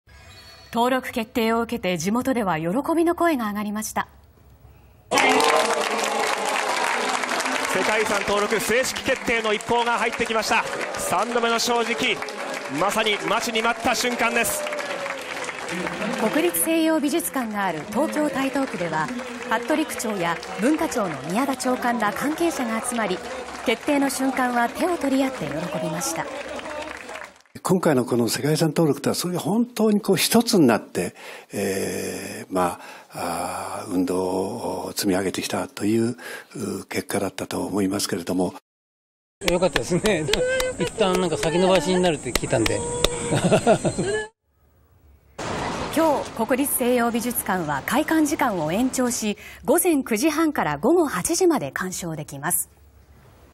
“世界遺産”決定に地元が歓喜 国立西洋美術館(16/07/18)
東京・上野にある国立西洋美術館の世界遺産への登録が正式に決まりました。登録決定を受けて、地元では喜びの声が上がりました。　国立西洋美術館がある東京・台東区では服部征夫区長や文化庁の宮田長官ら関係者が集まり、決定の瞬間は手を取り合って喜びました。